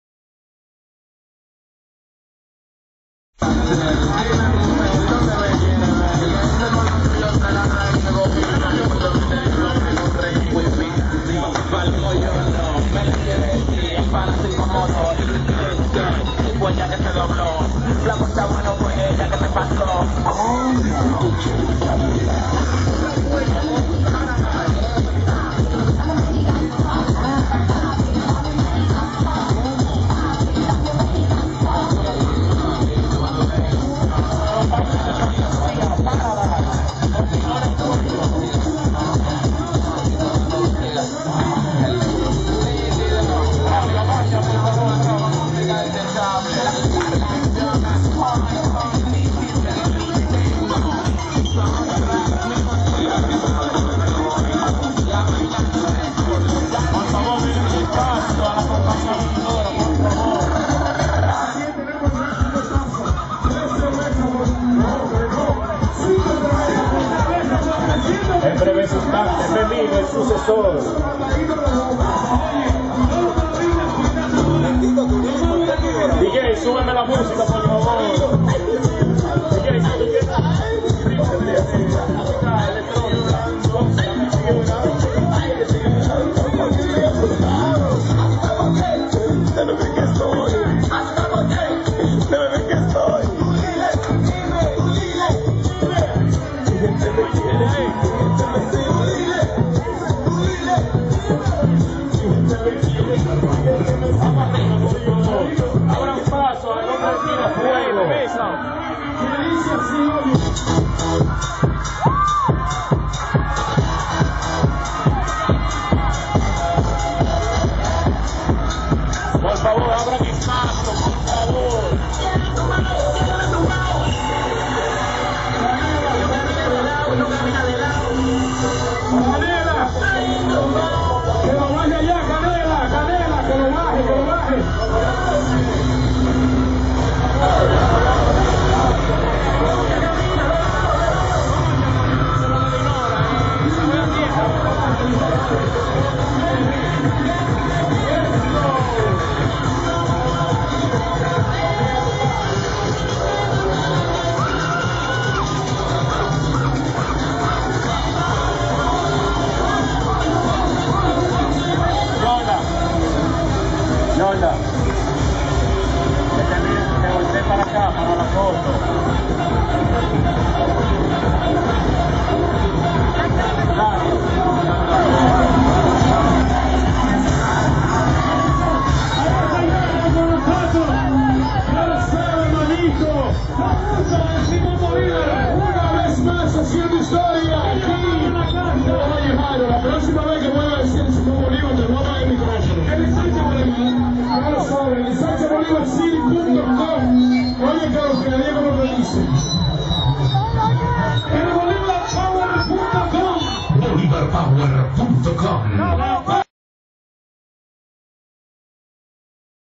Presentación Completa Del Grupo SABOR A TIERRA Representacion Del Ensanche Bolivar.
Con este grupo cerramos con broche de oro el desfile de carnaval de nuestro ensanche bolívar (El Mejor Carnaval De Santiago) Otra excelente presentación de una de las comparsas mas importante de nuestro ensanche bolívar, SABOR A TIERRA, dirigida por la señora DIGNORA PUELLO. Esta comparsa es de las que mas premios a recibido fuera de nuestra ciudad y siempre esta en los primeros lugares acaparando la atención de los lugares que asisten. Felicidades a cada uno de sus integrantes por su buena actuación tanto a Braulio Antonio Lopez y Nicole (Jordan) Martinez quienes dejaron a todo el publico sorprendido con sus tan vistosos disfraces.